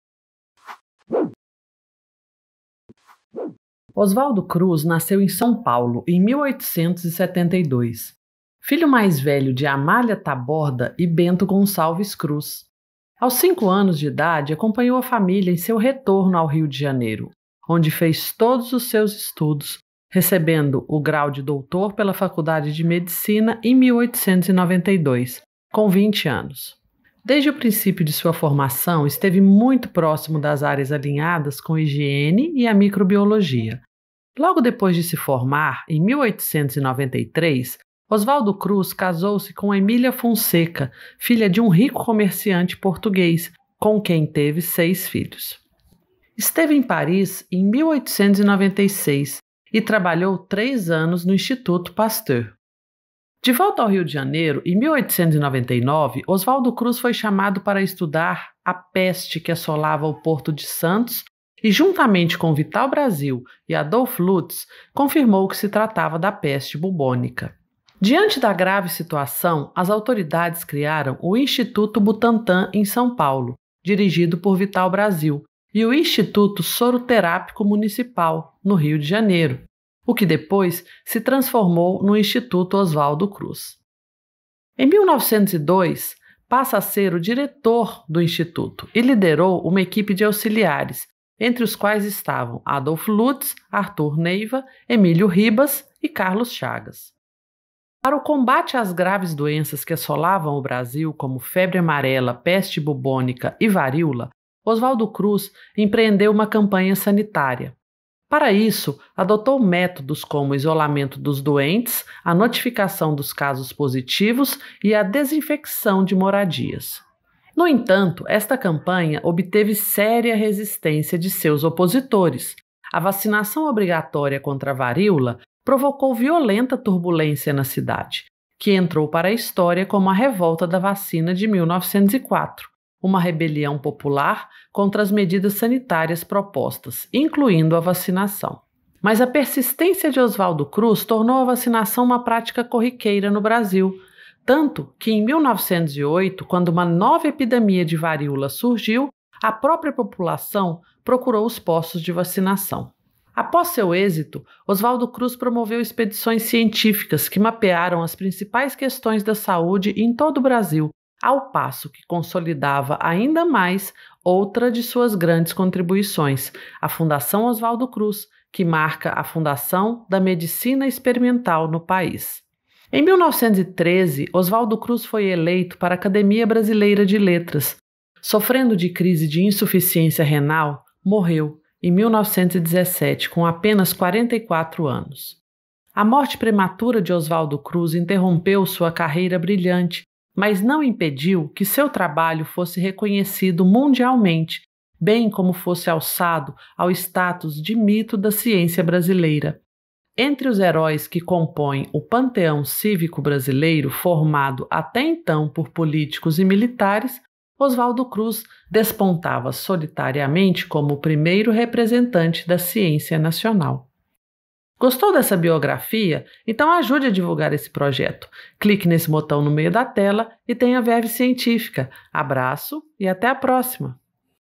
Oswaldo Cruz, O Pai da Vacinação em Massa no Brasil
Oswaldo Cruz é sem dúvida um dos maiores cientistas do Brasil. Com experiência e renome internacional, Oswaldo Cruz ajudou a estabelecer uma política básica de saúde no Brasil e suas ações foram cruciais para erradicação de moléstias e mudança de mentalidade no país no início do século 20.

* Este conteúdo teve a locução e a contribuição técnica e científica da Prof. Dra. Thaciana Malaspina (CV